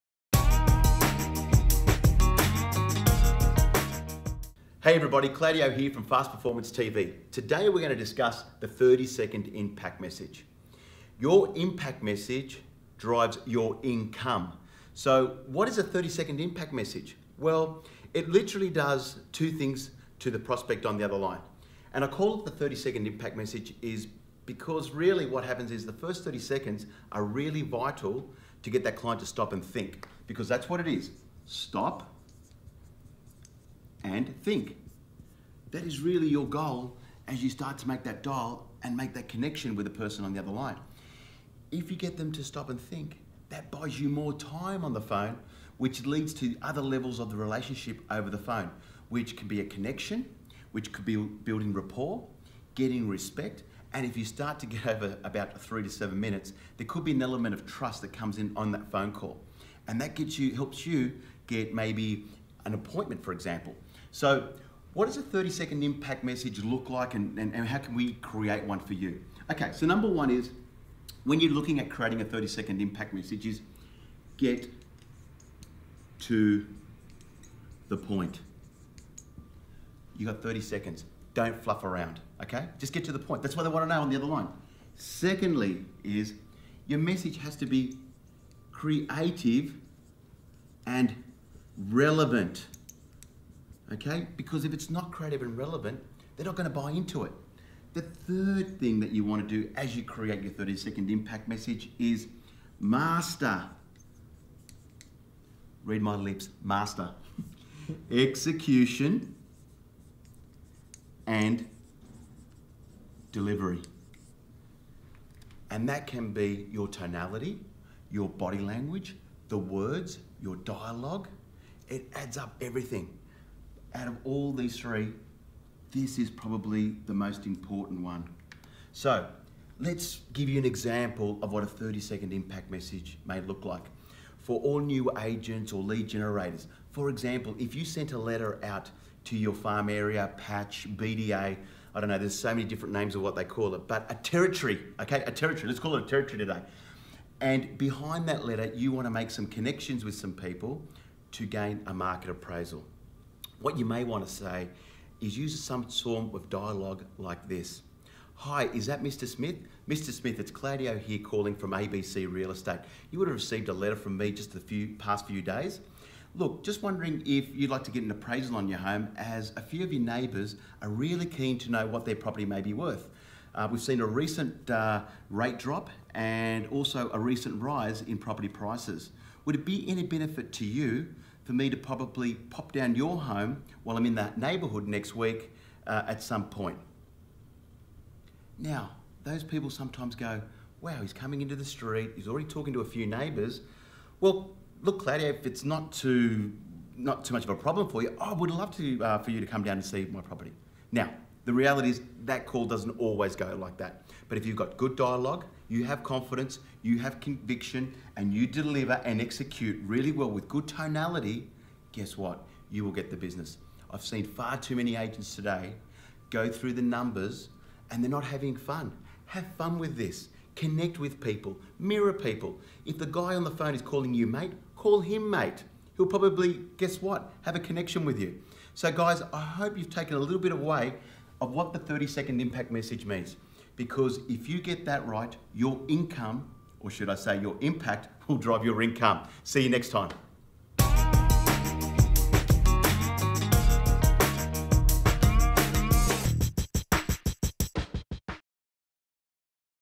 "Your 30 Second Impact Message" - Claudio Encina
Your Impact Message Drives Your Income

- Get your client to stop & think
- Create a relevant message
- Master your execution & delivery

Fast Performance TV